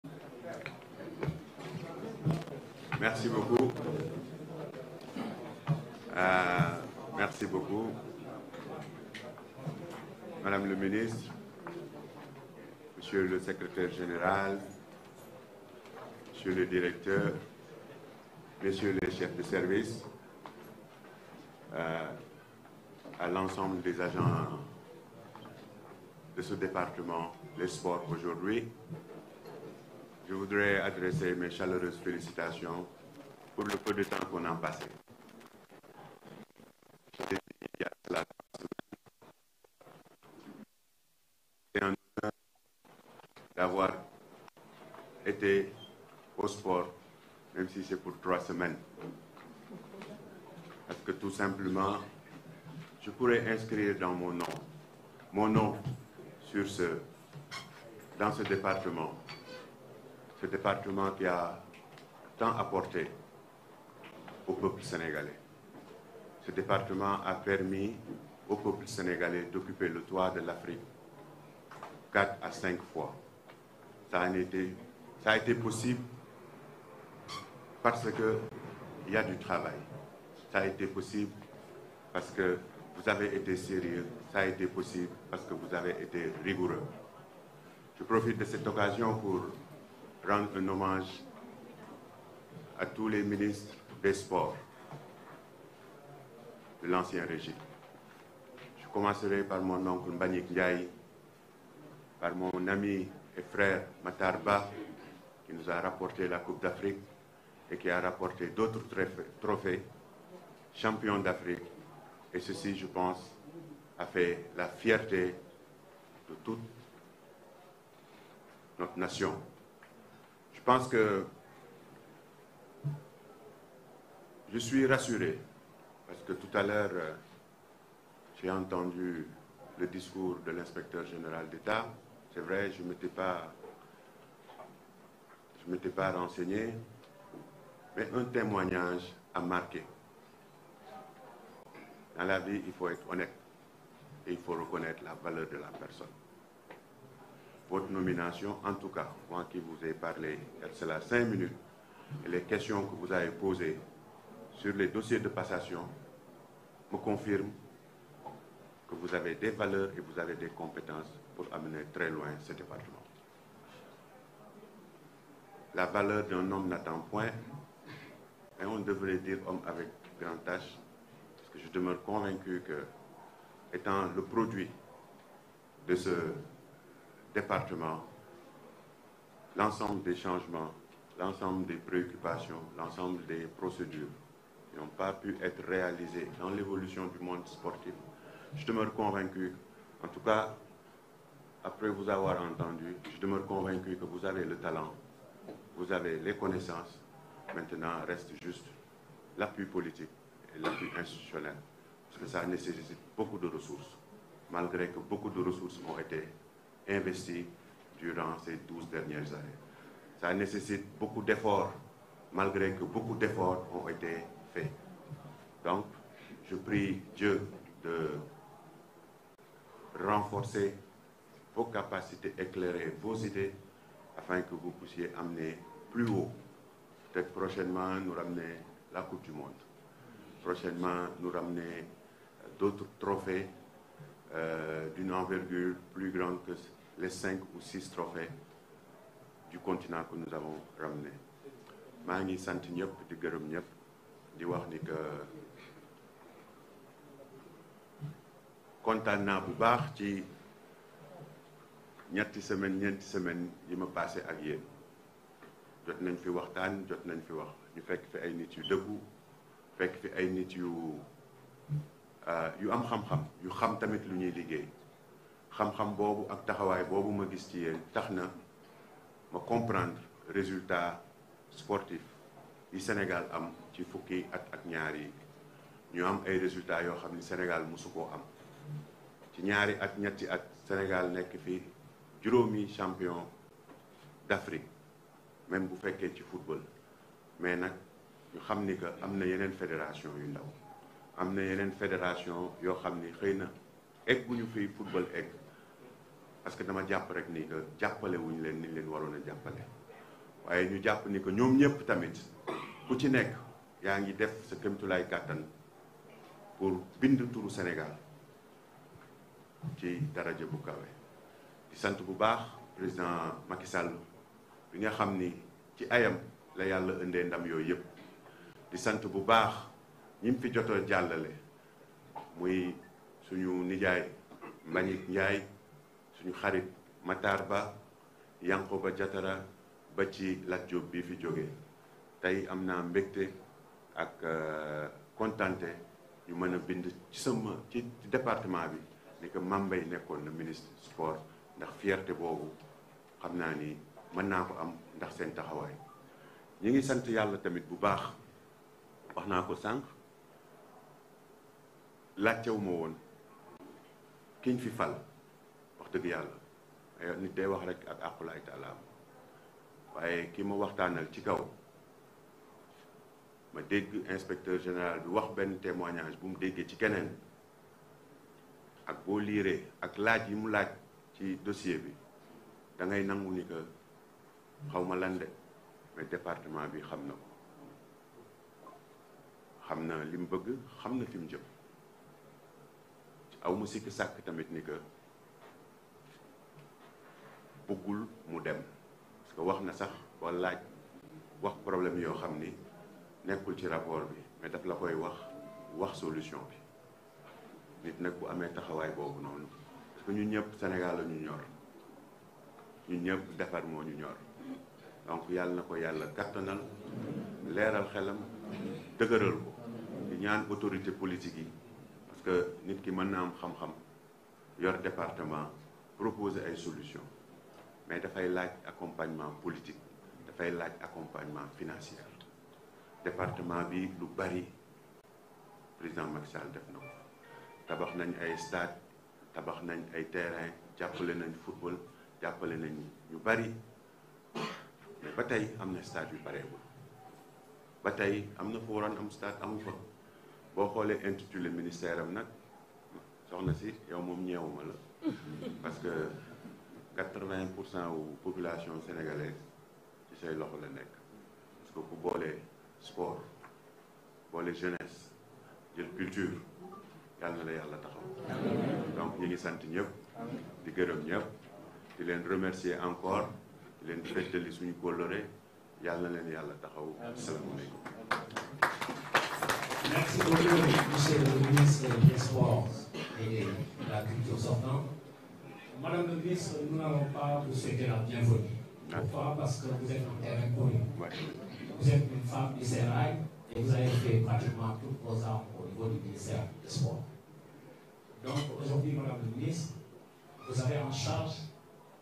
Témoignage Mame Mbaye au ministre khady Diene«kou xam lignko déink,yalla nga fi indi coupe du monde
🔴Direct Passation de service aux Ministère des sports entre Mame Mbaye Niang et Khady Diéne